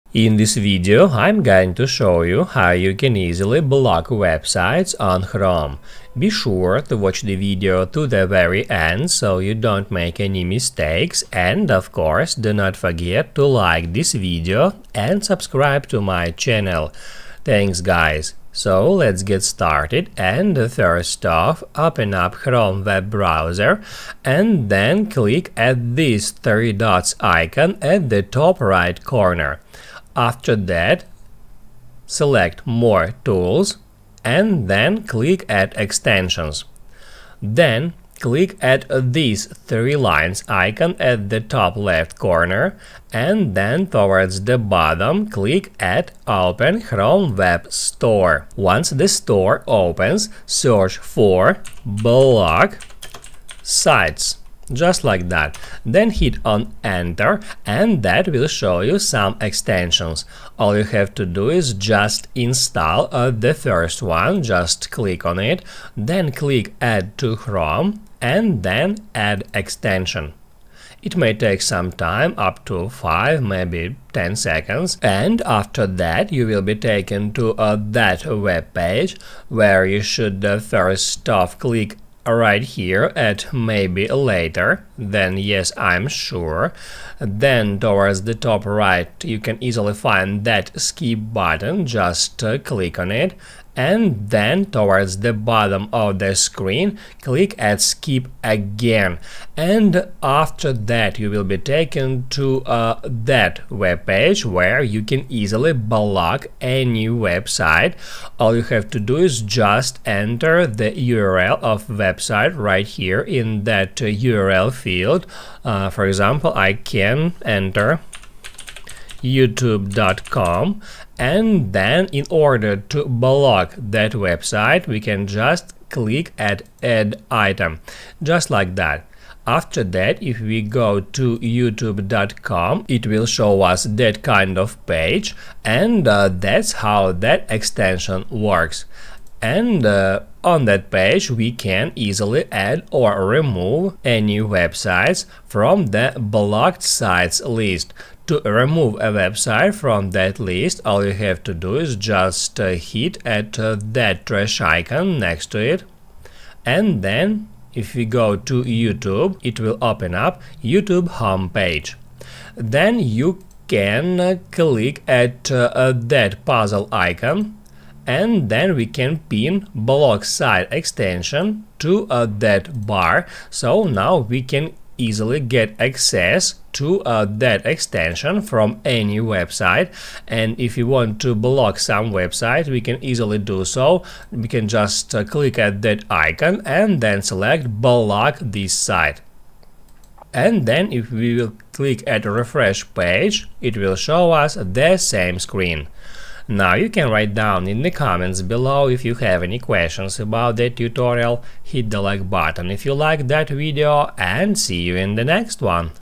How to Block Websites on Chrome - Full Guide
In this video you will learn how to block websites on chrome